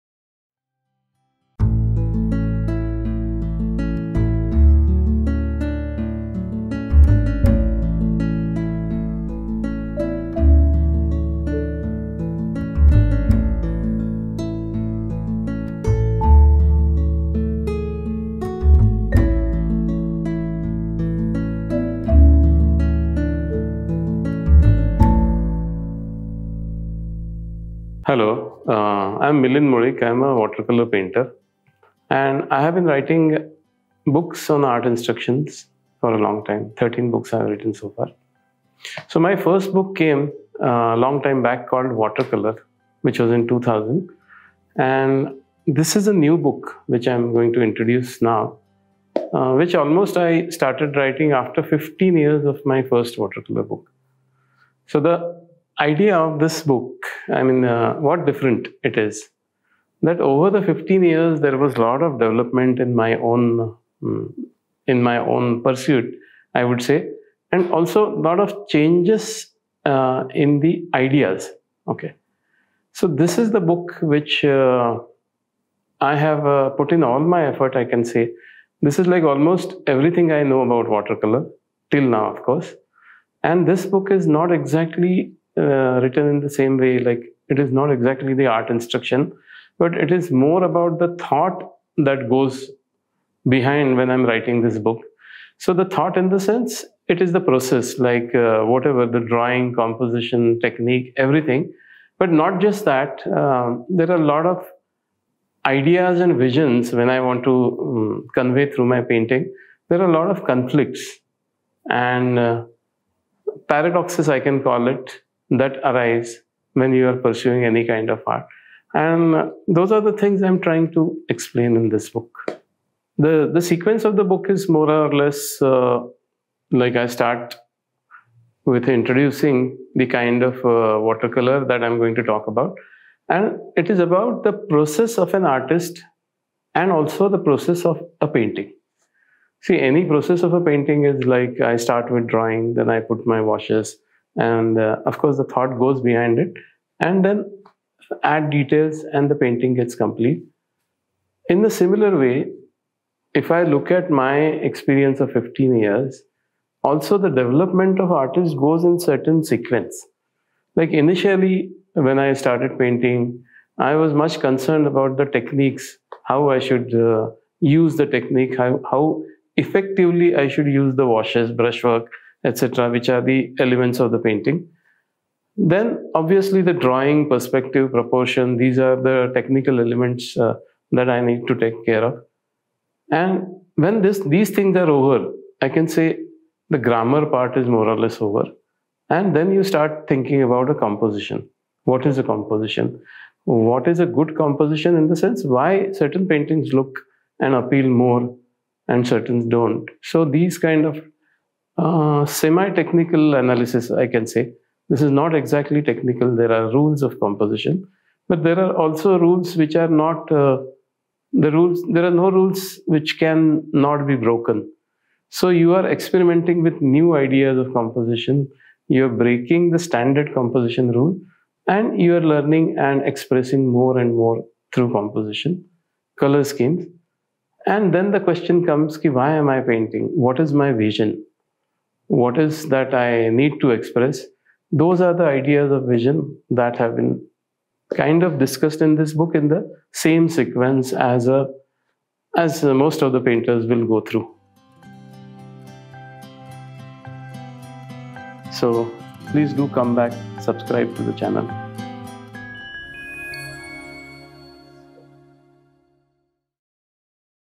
Introduction to 'Expressions in Watrcolour' by Mlind Mulick
About this book:
    This is the sequel to the earlier book on watercolour. It is a new, hardbound edition in smaller size, with some new paintings.
    After the botheration of technique is over, i.e., 'how' in watercolour is no longer a problem, many aspects of the painting process lay ahead.
    In this book, Milind Mulick talks about his journey so far, the techniques and thoughts behind the painting process, which he has pondered for a long time. He shares his thoughts through words and colours. He also wishes to discuss the many conflicts that keep arising while painting.
    This book will certainly help the budding artist to find his own way!